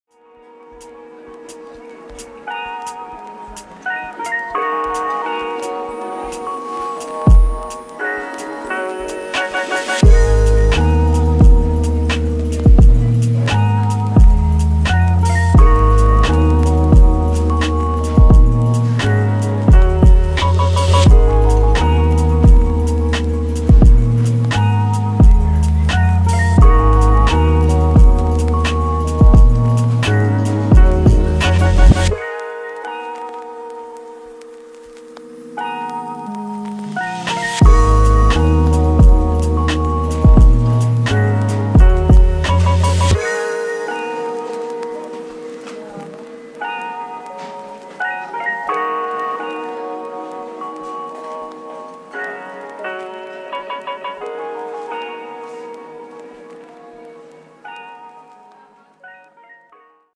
COBB Tuning - Coffee with Cars Dripping Springs, October 2019
We drove an assortment of COBB shop cars down to Greater Goods Coffee Roasters for the October Coffee with Cars-Dripping Springs event to announce the release of an Anniversary Blend coffee.